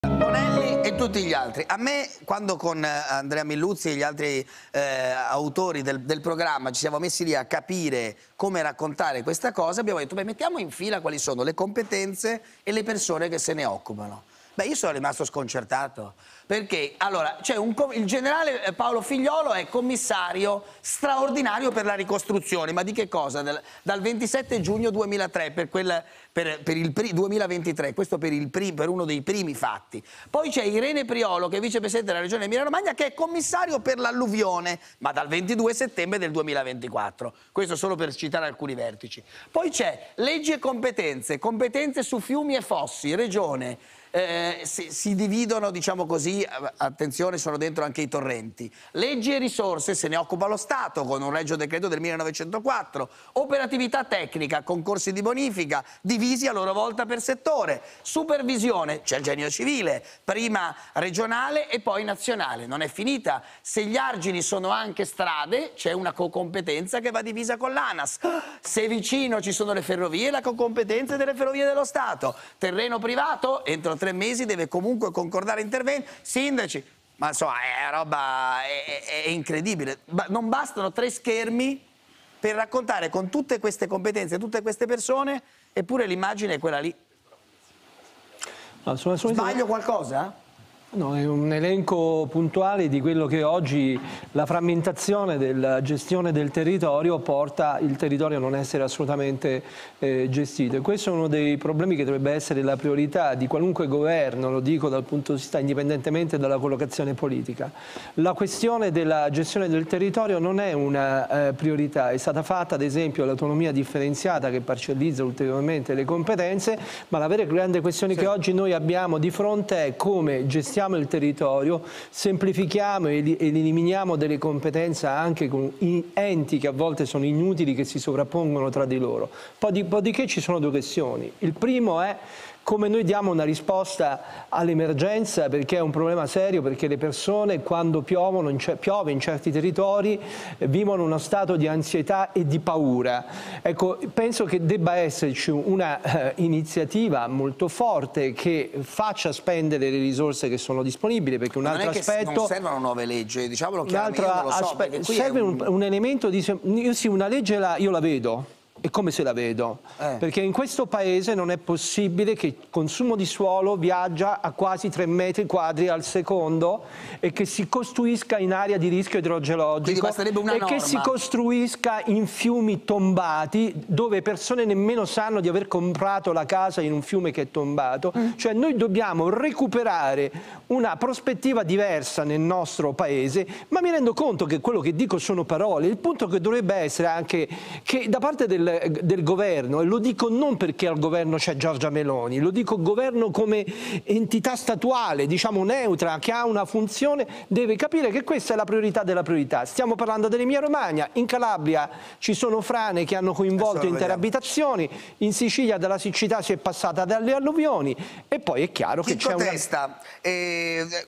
Alluvione in Emilia-Romagna, Angelo Bonelli: "La frammentazione della gestione del territorio ...
Angelo Bonelli (Alleanza Verdi e Sinistra): "La questione della gestione del territorio dovrebbe essere la priorità di qualunque governo. È stata fatta l'autonomia differenxiata, che parcellizza ulteriormente le competenze, ma serve semplificare ed eliminare enti inutili. L'emergenza è un problema serio".